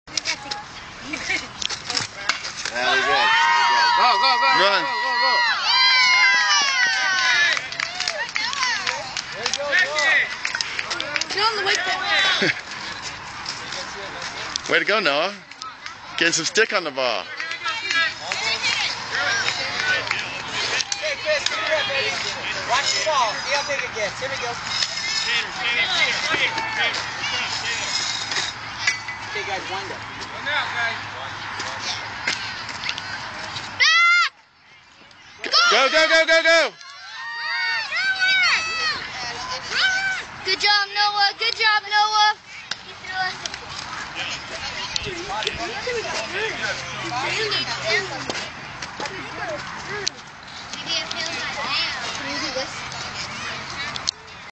Noah gets 2nd of 2 hits on 10/21/09
this is his 2nd hit - also a nice shot into the outfield, he also then steals 2nd base when they try to pick him off of 1st